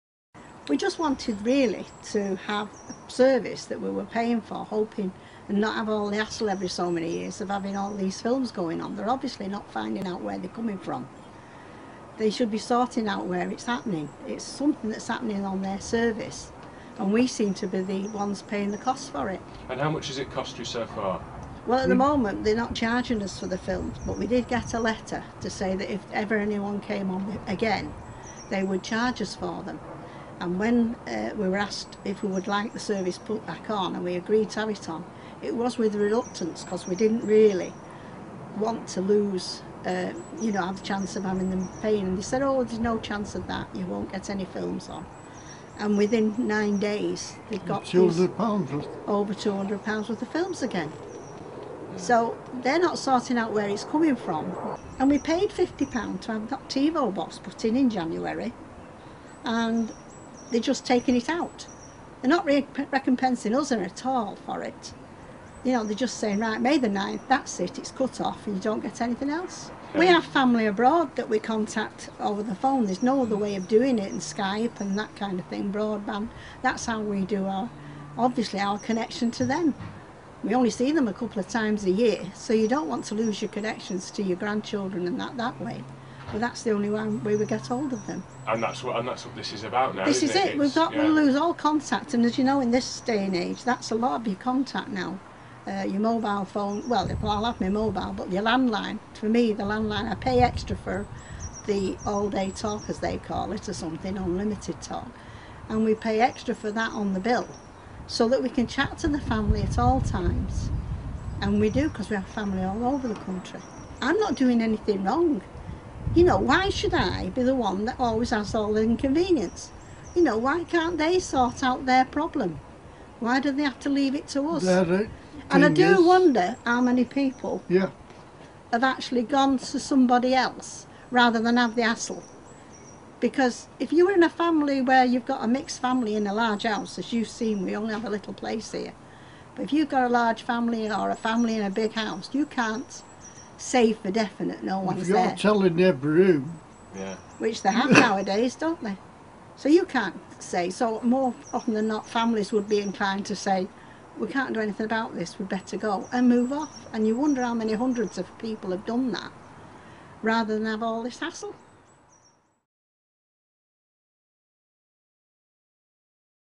Elderly couple Virgin porn bill interview
A couple has been handed a £900 bill from Virgin Media for pornography over a five-year period - despite the fact that they insist they have never ordered any.

Ann, 72, and Ron Hayward, 75, who live in a one-bedroom bedsit in Stockport, Cheshire, now have no internet or television because the provider have accused the couple of 'abusing' services.

The great-grandparents first received a bill for almost £200 in 2009, and another £200 last week. They were also sent another letter ordering them to pay more than £500 for blue films.

Adult channels paid for through Virgin media cost £5.99 for a 16 hour period.

Users can also buy a monthly subscription package to channels such as Playboy TV for a monthly fee of £11.

For the price of the bill, the couple could have watched 150 16-hour stints through different channels or a total of 2,404 hours of pornography - the equivalent of 100 days.

But the pensioners, who live in a quiet housing block for elderly residents, were adamant they had never ordered, let alone watched, the pornographic films.

The company then agreed to waive the bills and said they would remove the ‘on demand’ feature.

It was not until they upgraded their service to Virgin’s latest 'TiVo' box last month that they reluctantly agreed to reactivate their on demand services, and problems resurfaced.

The TiVo box allows viewers to record up to 500 hours of television.

Mrs Hayward, 72, said: 'Because of what happened before, we were reluctant to have the on demand services switched back on - even though we were paying the full cost for them the entire time.

'The lady I spoke to promised me that we had absolutely nothing to worry about and that there was no way it could happen again.

'After a couple of days a message appeared on the TV saying ‘channels unavailable’ and I had a feeling something wasn’t right - that’s one of the things that happened the last time.

'I started to get a bit worried so I called them the next day, just to be told that I had ordered more than £200 worth of films.

'I was in complete shock - I just couldn’t believe it was happening to us all over again after they reassured us that everything would be okay.'

Last week, the elderly couple received another bill for watching dozens of 18-rated films a day - costing more than £200.

Virgin staff have now told the Haywards that all of their services will be cancelled because ‘they are abusing the service’ - and they have until 9th May to find a new provider for their telephone, broadband and TV.

Retired nursery nurse Mrs Hayward said: 'I told them I’m not paying a penny of these bills - they insist it is us, but they won’t even tell us what time the films have supposedly been downloaded.

'I just feel like I am being ignored and it makes me so mad that this big company can just brush off their customers like that.

'Just because they can’t find a solution to the problem they are going to cancel all of our services - it is so stressful and frustrating for us.

'We are retired and in our 70s, we can’t afford to pay out to have everything installed again.'

The pair has a large family with five sons, 12 grandchildren and four great-grandchildren but both Mr and Mrs Hayward are adamant that no one else has had access to the television.

To watch adult films through Virgin, the user has to enter an adult pincode.

Mrs Hayward added: 'We live alone in a small bedsit, in a secure residential complex - no one else has access to our flat.

'If we lived in a big house with a big family then maybe I could understand it, as you couldn’t know what was happening in every room.

'You wouldn’t believe the conversations we have had to have with our family, friends and neighbours about it all.

'There is obviously something wrong with the technology or someone somewhere is hacking into our services, and if it can happen to us it can happen to anyone.

'Virgin just keep insisting that the films have been bought using our box and that it must be us doing it. But it isn’t as if someone has been sneaking in and out of our flat.

'The whole situation is just ridiculous. I am just completely gobsmacked, they were supposed to have made the situation better for us, but they have just made it worse.'

Dana Hashem, from Virgin Media, said: 'These films were unquestionably watched by someone in the Haywards’ home.

We are happy to arrange a payment plan but have said they may want to find an alternative provider going forward.'